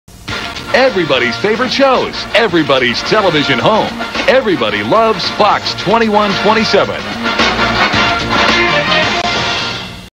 WJPR/WFXR Station ID 2004 #2
This is WJPR-WFXR's second station ID from 2004, which uses the graphics for Fox's 1998-99 image campaign "Just One Fox"
WWCW is located in Lynchburg, VA
WFXR is located in Roanoke, VA